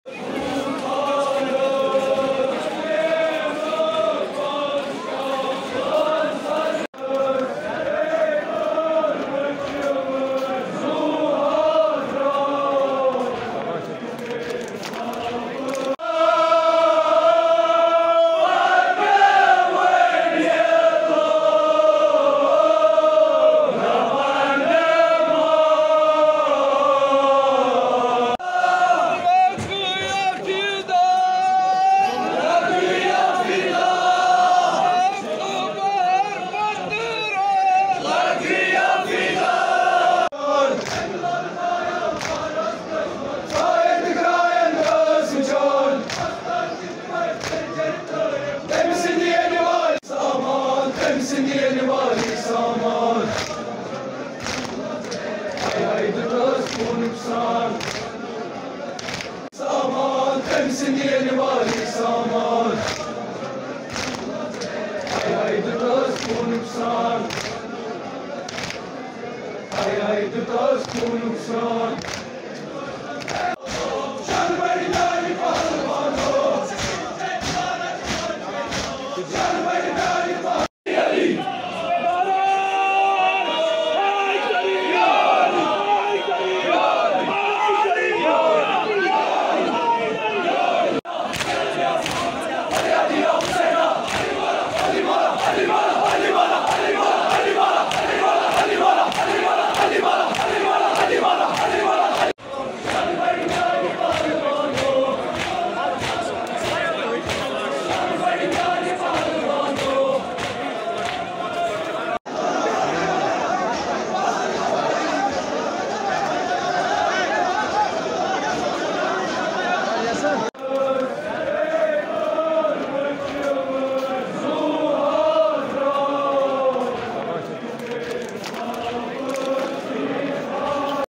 6th Muharram Procession Held in Srinagar's Kamangarpora
6th Muharram Procession Held in Srinagar's Kamangarpora

Report: Waris Nissar